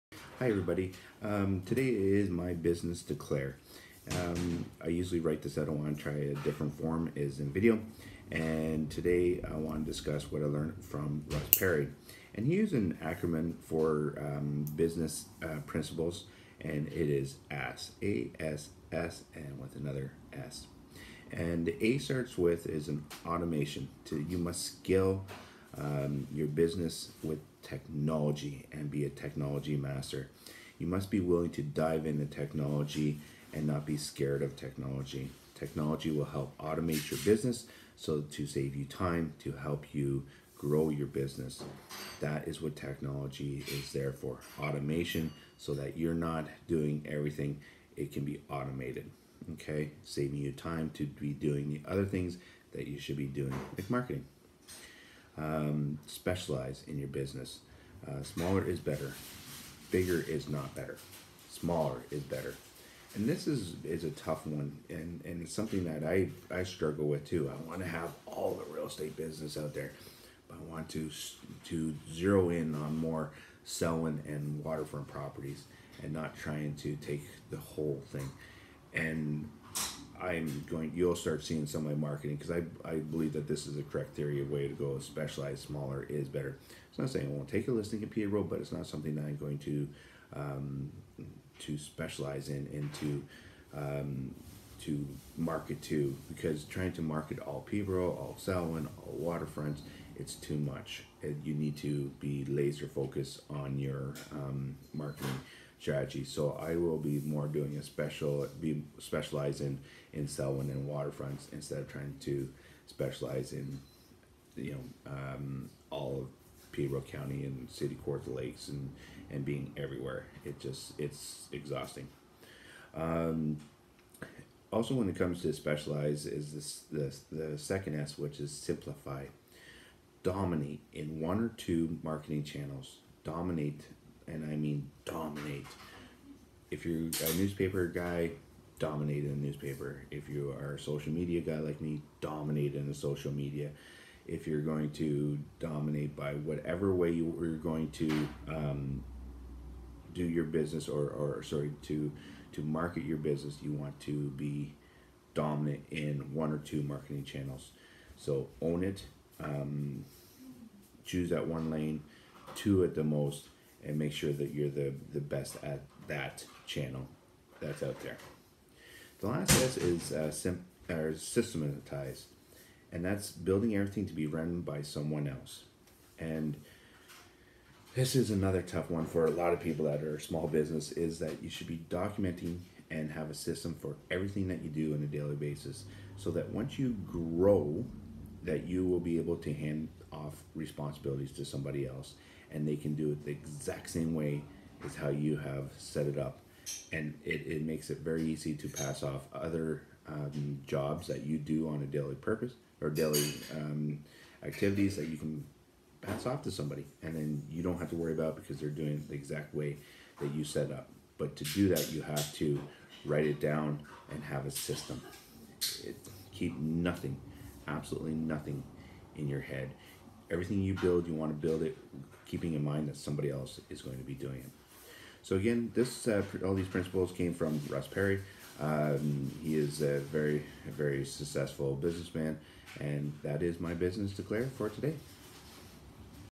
Brad Sinclair Business Declare, what I learn is what I teach
Business Declare: Now I am teaching what I learned this morning via video. I learn more and so do you!

Todays Principle from Russ Perry
A.S.S.S. Business Principles